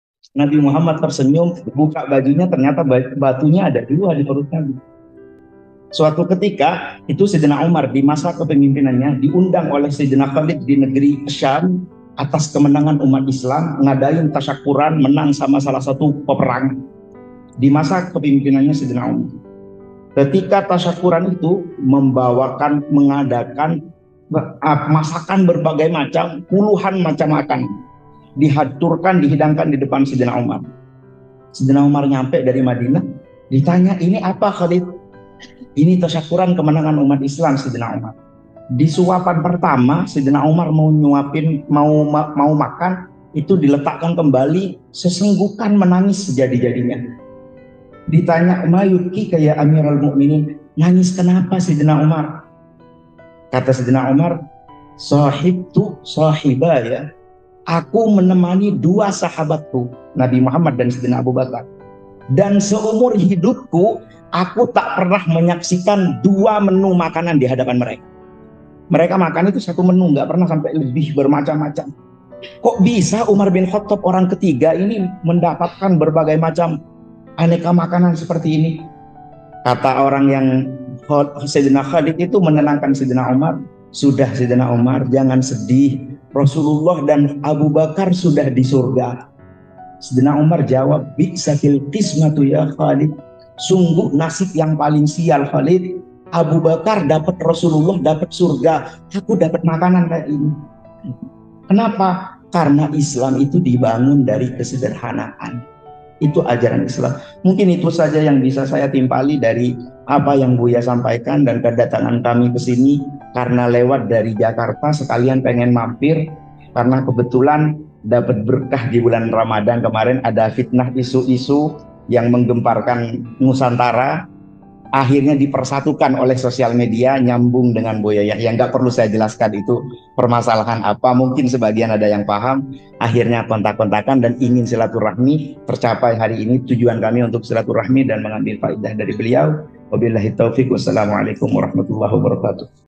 Sowan buya yahya di albahjah❣️
Kemarin seusai dari jakarta menyempatkan sowan pada buya yahya, ternyata di ponpes albahjah sedang ada pengajian rutinan, dan spontan buya yahya mengajak kami ke majelisnya, menyimak kalam indah beliau selama satu jam, sangat bersyukur menimba ilmu dan faedah dari beliau, namun tanpa diduga di akhir majelis beliau men3mbak alfaqir untuk berbicara beberapa menit, malu sekali rasanya namun juga haru bisa dipercaya oleh para ulama shalihin meski alfaqir jelas-jelas masih jauh dari kata pantas, usai pengajian dijamu masha allah dan menyempatkan diri untuk mengambil ijazah dan sanad tentang banyak hal, Alhamdulillaaah... dan saya melihat sendiri kesederhanaan buya yahya itu nyata, kalau makan masak sendiri ngulek sambel sendiri, memang begitu para kekasih Allah swt sebanyak apapun hartanya dan selapang apapun rezekinya, mereka selalu merasa malu untuk mengangkat kepala jumawa, Semoga keberkahan para kekasih Allah dari para habaib dan para kyai serta para hamba pilihan yang Allah rahasiakan, senantiasa mengguyur negeri ini dengan kedamaian. -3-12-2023 albahjah cirebon-